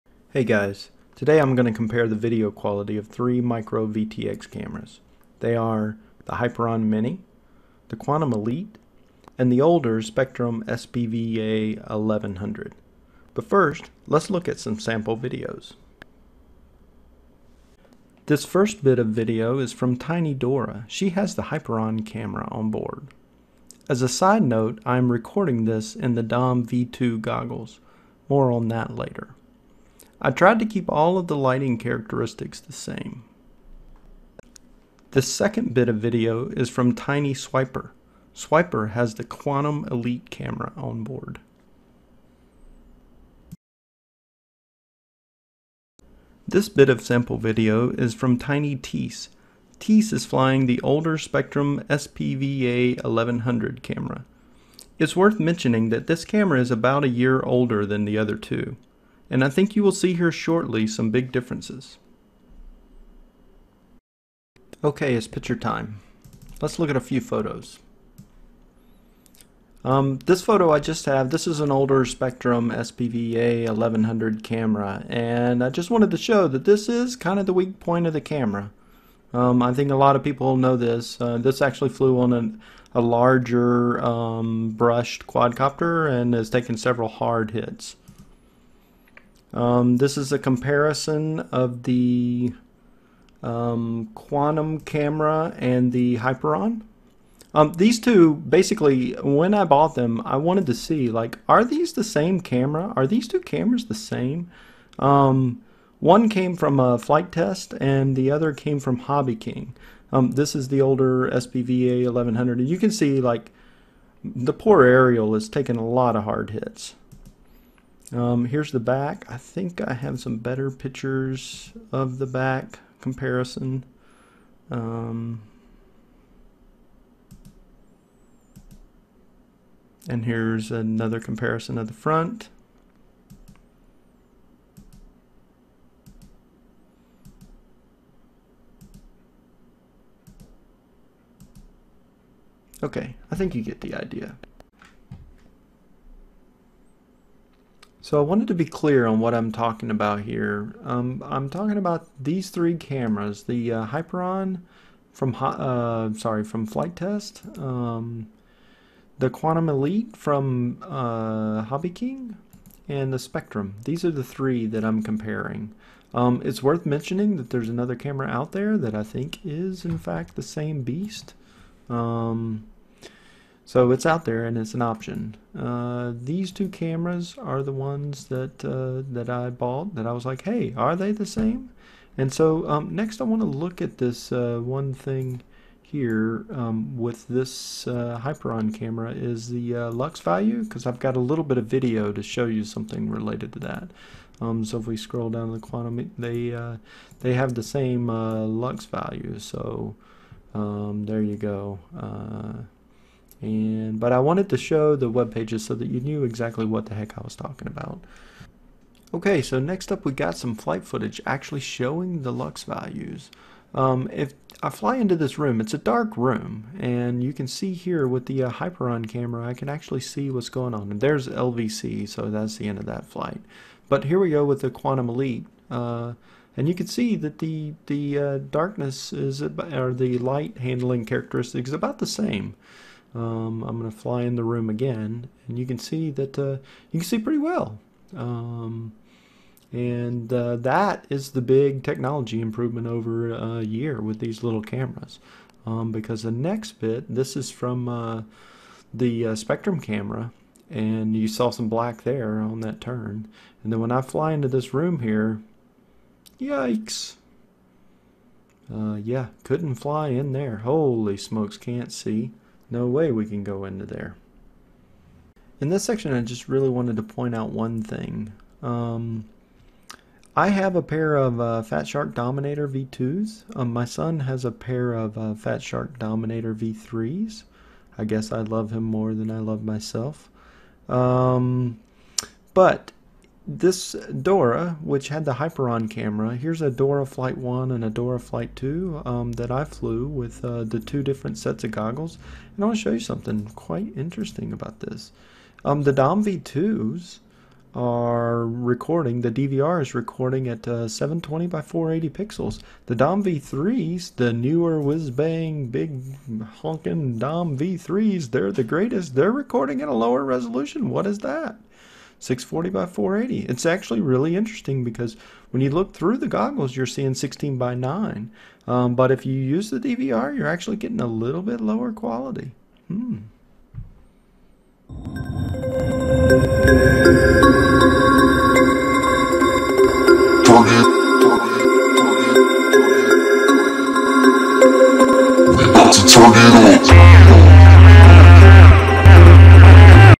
Tiny Whoop Video Quality Comparison
Here I am going to compare micro VTX cameras and see what there quality differences are.

The cameras: Best quality  ( I think these 3 are the same )

The cameras: lower quality ( worse 'low-light'  handling )

Other camera's popping up in the marketplace:
( I'm gonna list it here, because everything is out of stock )